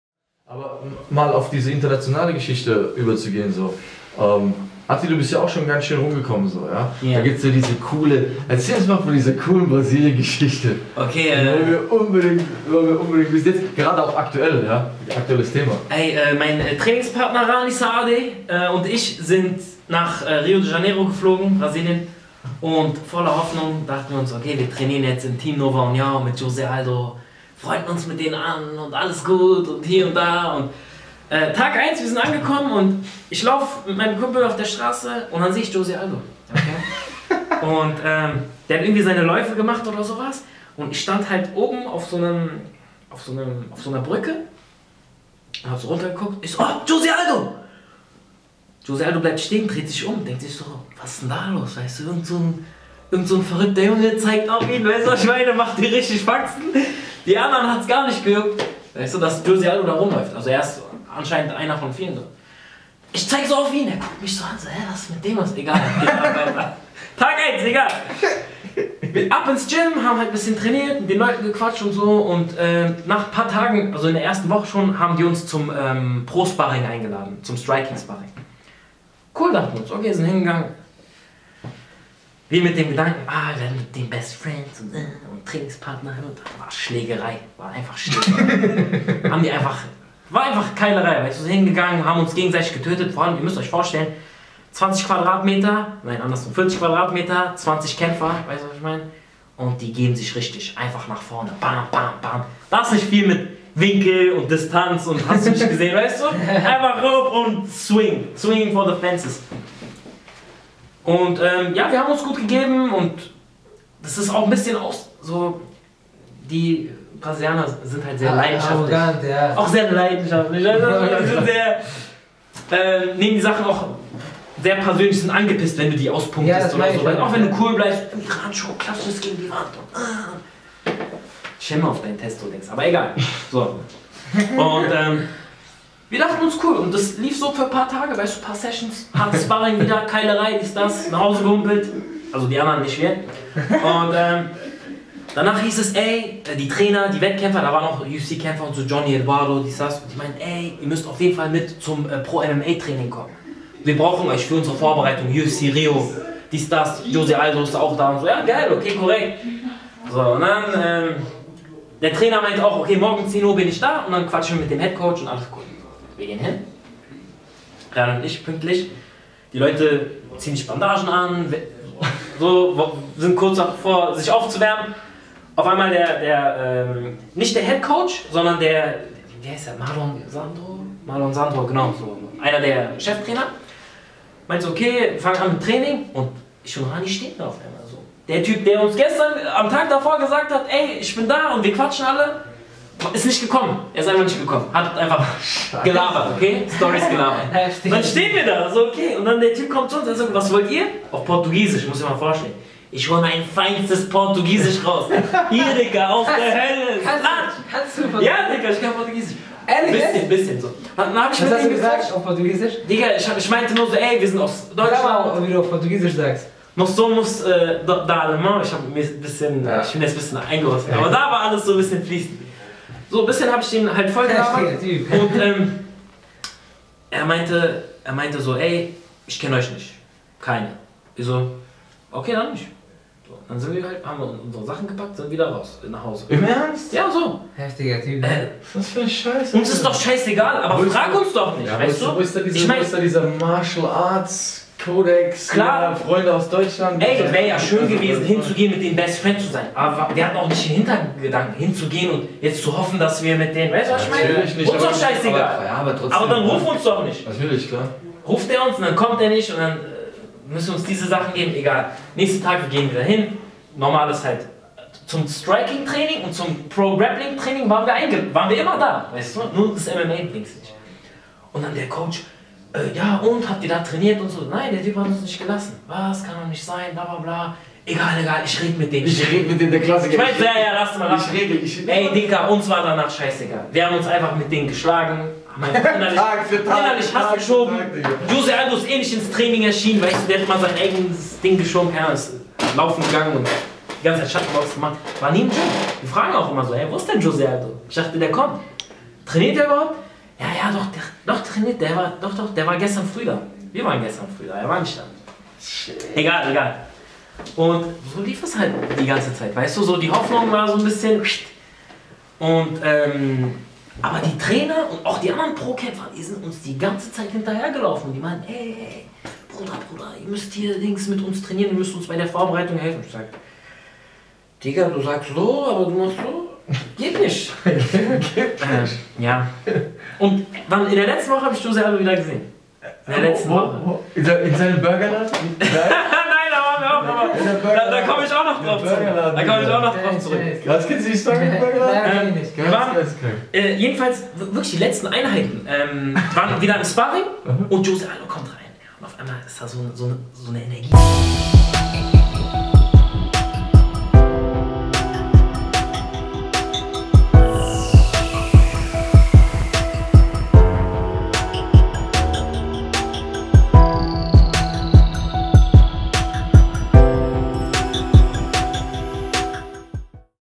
Choke Talk: Stories from Brazil with Nasrat Haqparast and Attila Korkmaz
We sat down in Berlin's Sony Center with UFC Fighter Nasrat Haqparast and "The fearless" Attila Korkmaz who most recently fought for ACB.
We had a nice Chat about their experiences during training camps overseas. make sure not to miss this one!